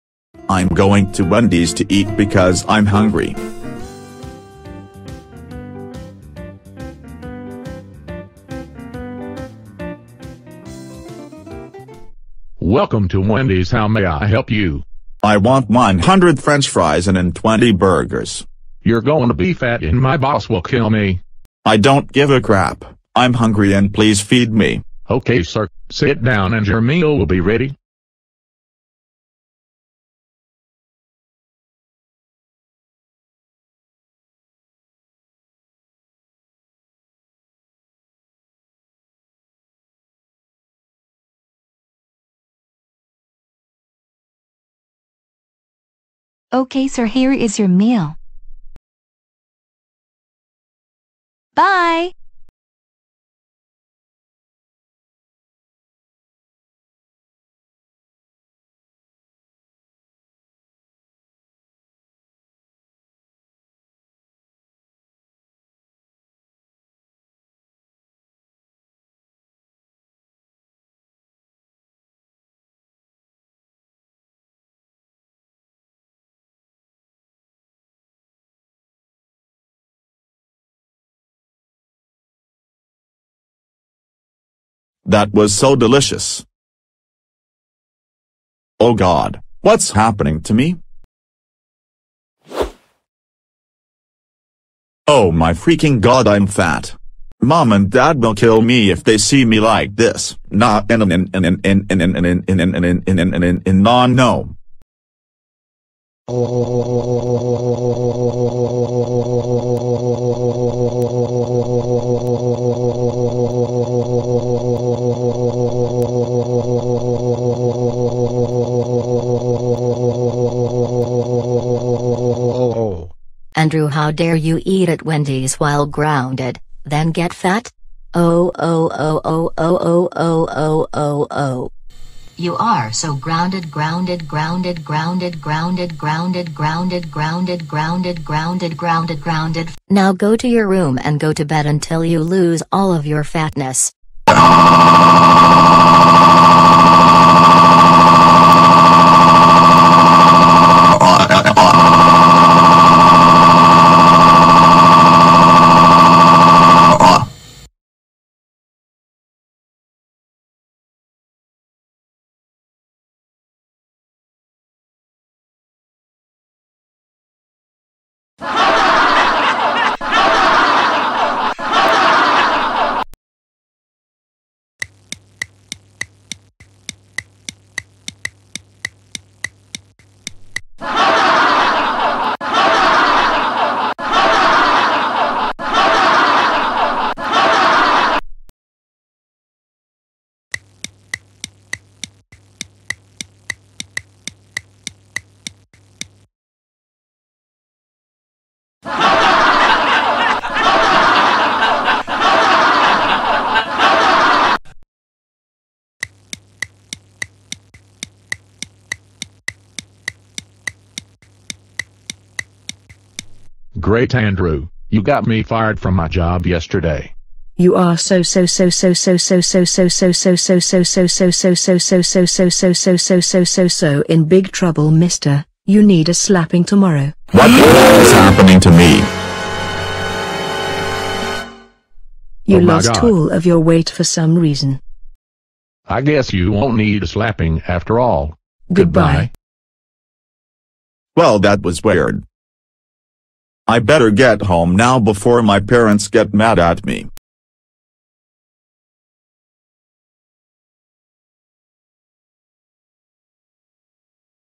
Andrew gets fat at Wendy's (Nakamario27)
This video has some realism and has more to the video
rather than the video ending with the kid running &amp; crying to his/her room.
No more Andrew grounded videos until next week.